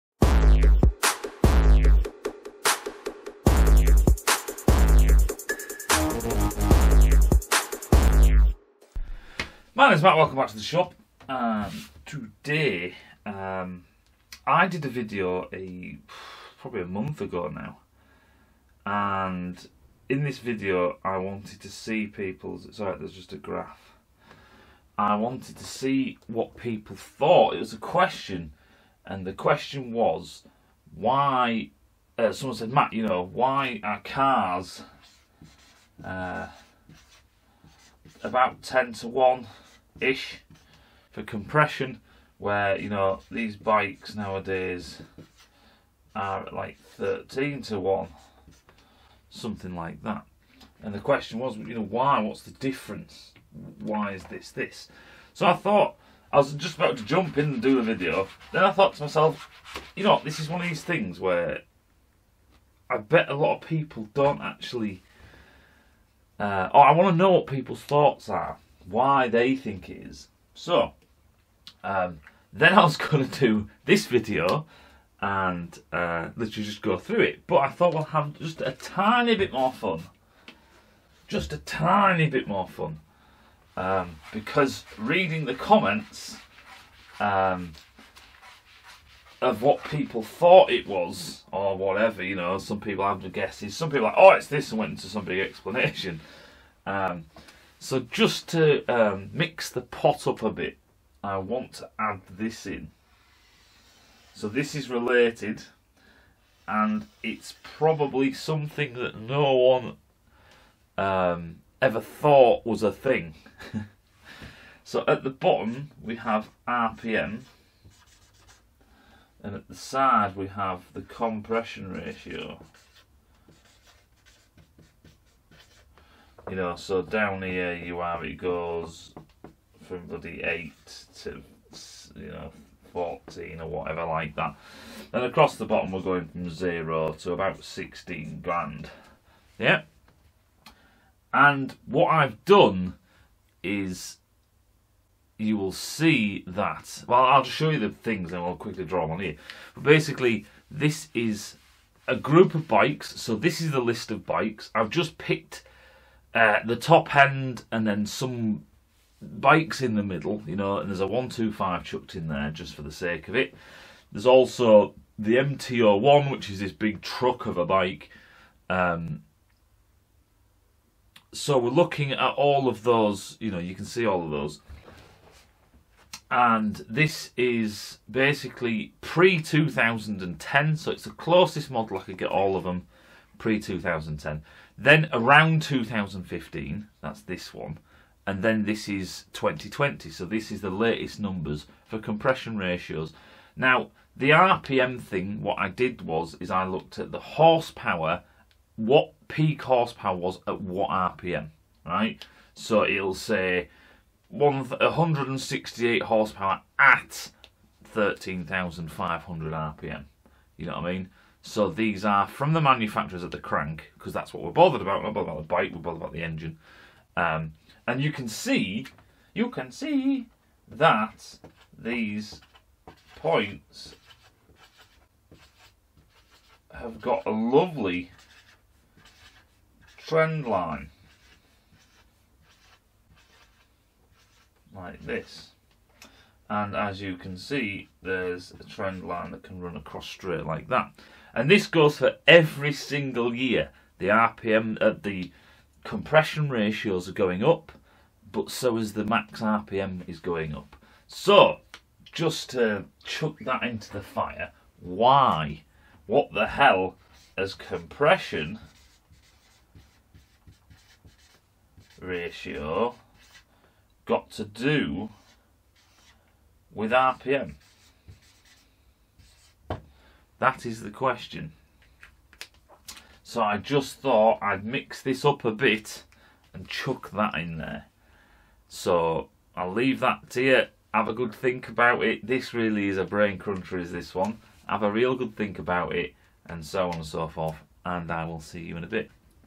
High compression and RPM?
What does RPM and Compression ratios have in common? Do they have anything in common? What the hell is going on?

CAD Models link -